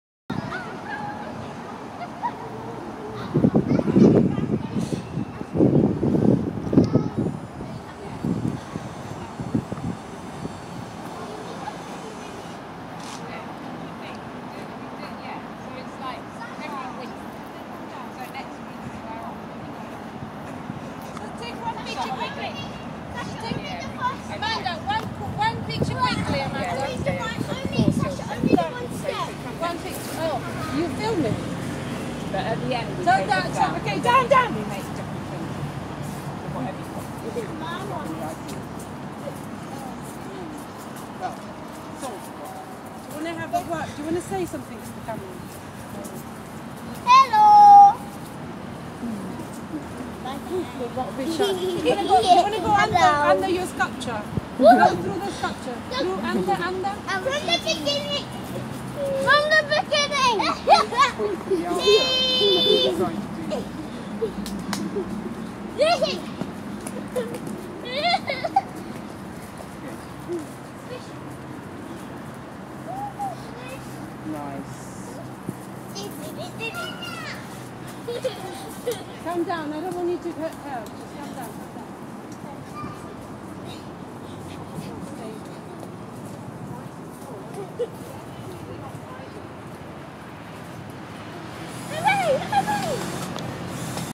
Big ZiG-Zag Sculpture
On week 5 Making Works session we built a giant zig-zag Sculpture to play with.